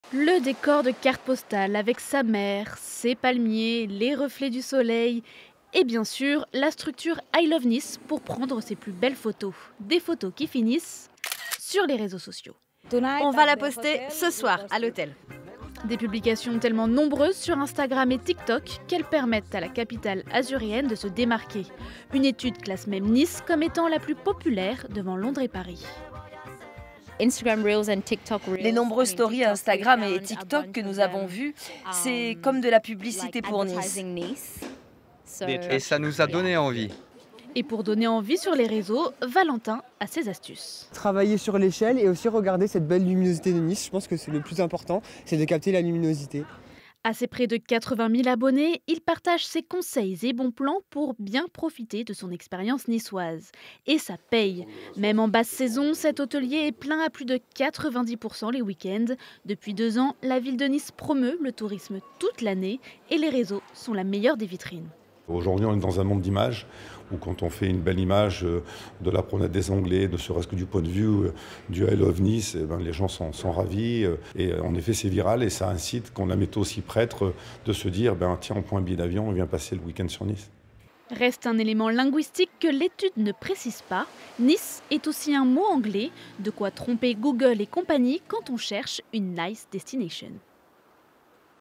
Nice détrône Londres et Paris sur les réseaux sociaux, les raisons de son succès
Parmi les destinations les plus convoitées pour partir en vacances, Nice se classe numéro un mondial. Selon une étude qui prend en compte la popularité des destinations sur les réseaux sociaux et les recherches Google, la capitale azuréenne est la ville touristique qui fait le plus rêver.